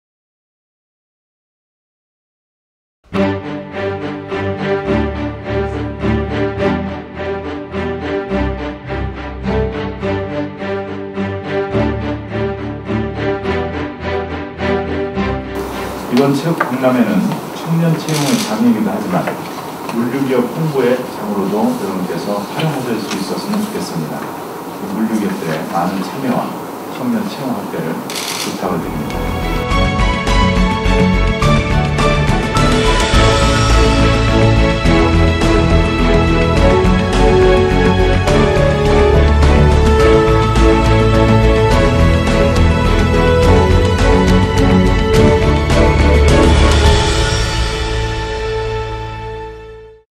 동남권 물류단지 현장 방문
제목: 동남권 물류단지 현장 방문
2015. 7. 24. / 서울 송파구 장지동

(자막 / 여헝구 제2차관, 동남권 물류 단지內 업계 의견 청취)

(자막 / 여형구 국토교통부 제2차관 / 이번 체험 박람회는 청년채용 작용도 하지만 물류기업 홍보의 장으로도 여러분께서 활용할 수 있었으면 좋겠습니다. 물류협회의 많은 참여와 청년채용 확대를 부탁드립니다.)

(자막 / "육ㆍ해ㆍ공 물류 채용박람회" 물류기업과 공공기관 약 60개사 참여)

(자막 / 다채로운 현장경험, 물류비전 공유 등 인력양성사업 한단계 더 발전할 것으로 기대)

(경제혁신 3개년 계획 3년의 혁신, 30년의 성장 / 광복 70년 위대한 여정 새로운 도약 / 국토교통부)